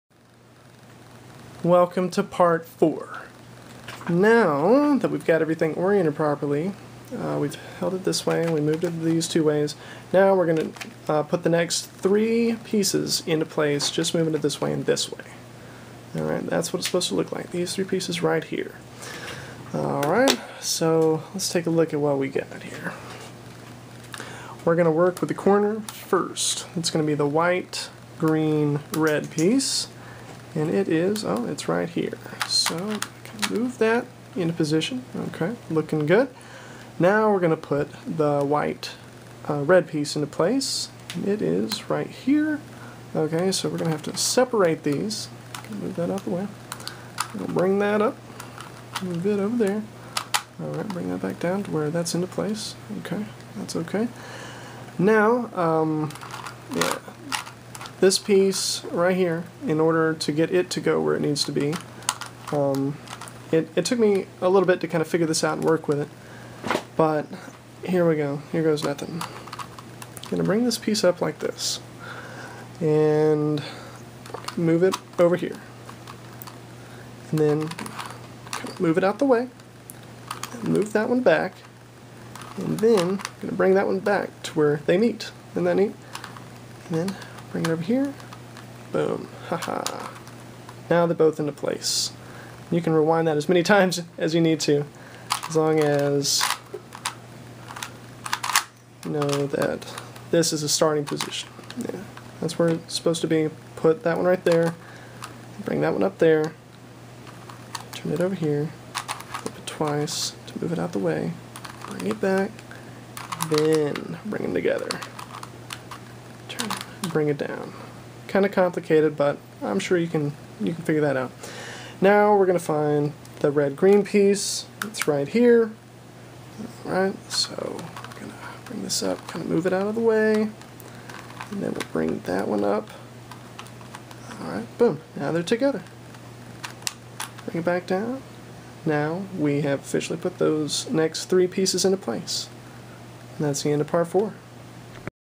134 - How to Solve Rubik's Cube Logically Part 4
Now we put the next 3 pieces into place.